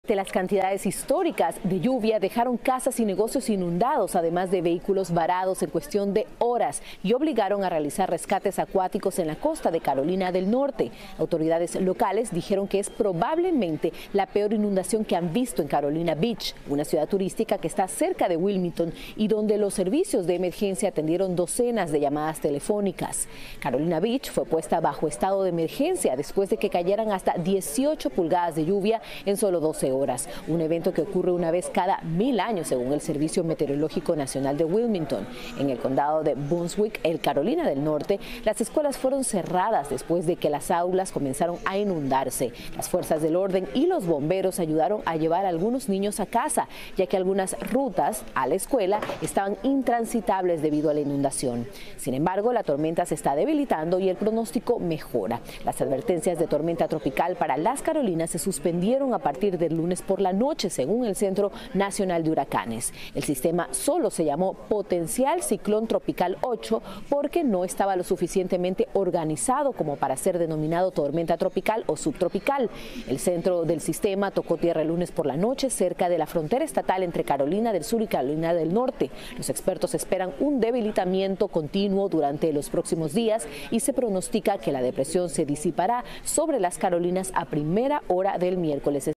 Sistema tropical provoca inundaciones sin precedentes en Carolina del Norte
Un sistema tropical, denominado como potencial Ciclón Tropical Ocho, dejó graves inundaciones en Carolina del Norte. El pueblo de Carolina Beach fue puesto bajo estado de emergencia tras registrarse hasta 18 pulgadas de lluvia en solo 12 horas. Por su parte, el Servicio Meteorológico Nacional de Wilmington dijo que un evento así ocurre una vez cada mil años.

Maity Interiano